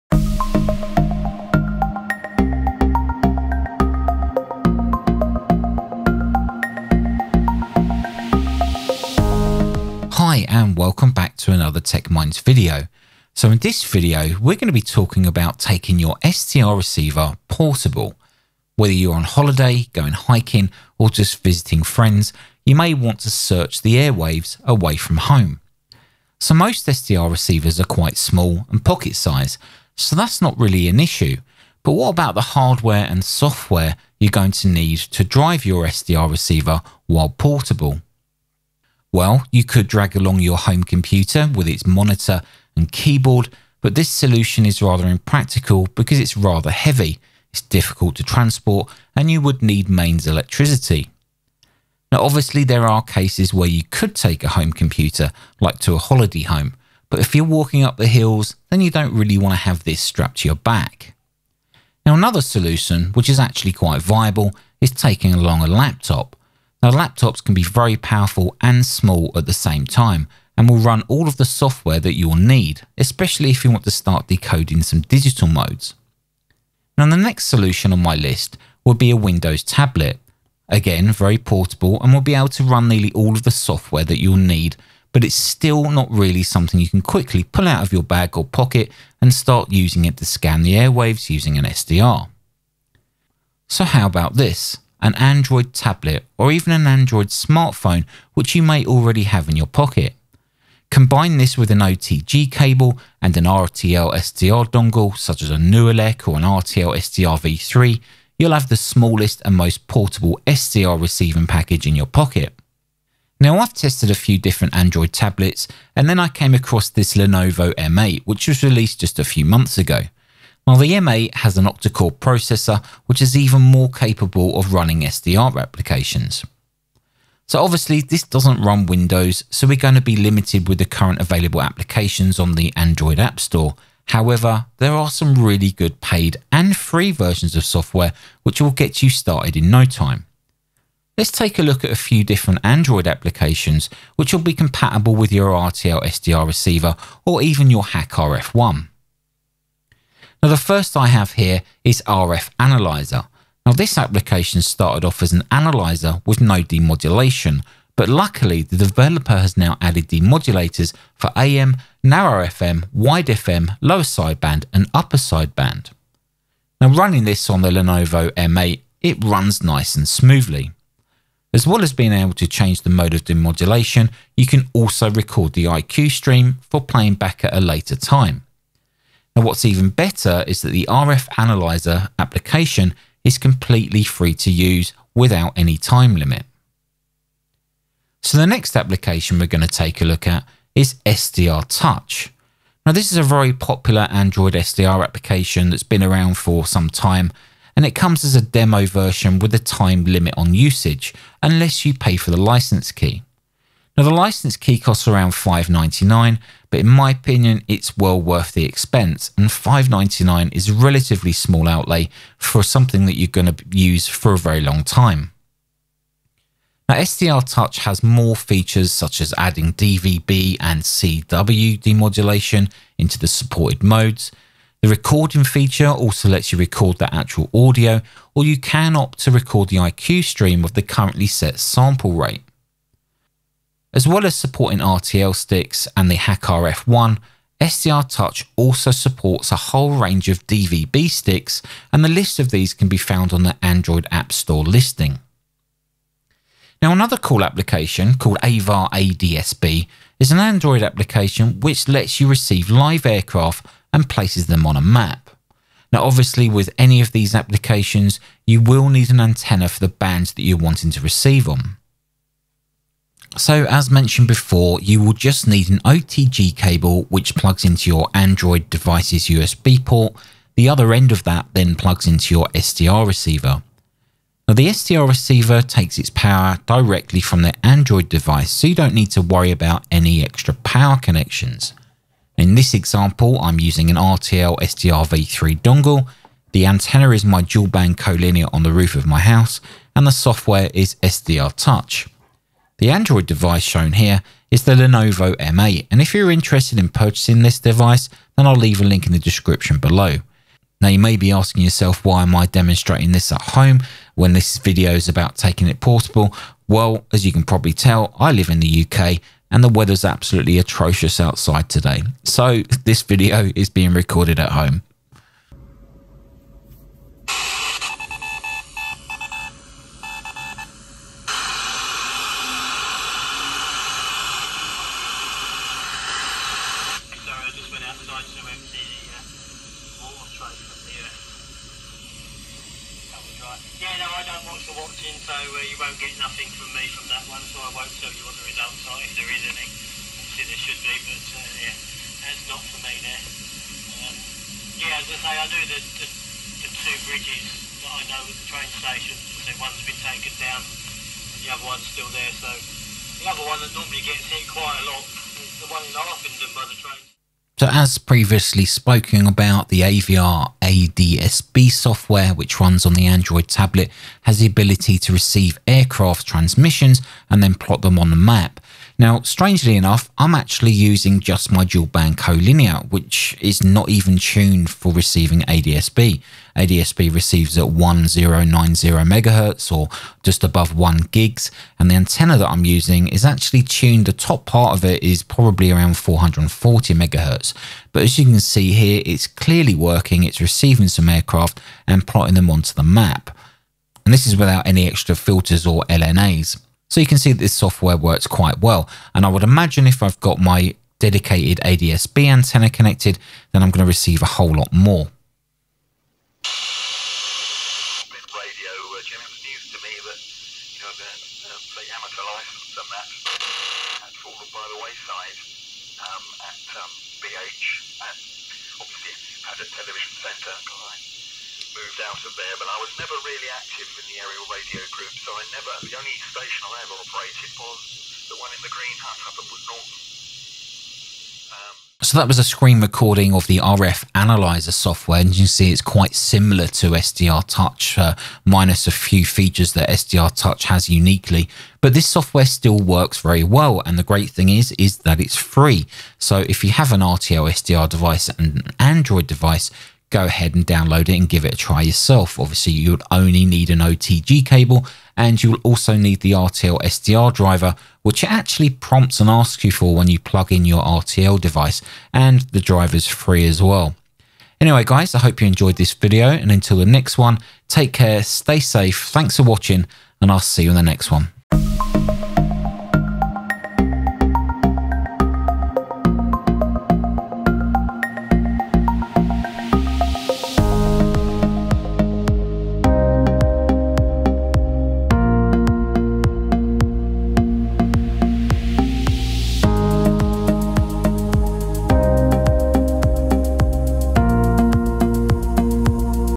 Portable RTL - SDR Software Defined Radio with Android
Here we take a look at possible hardware used for Portable SDR use.

Lenovo M8 Octacore Android Tablet: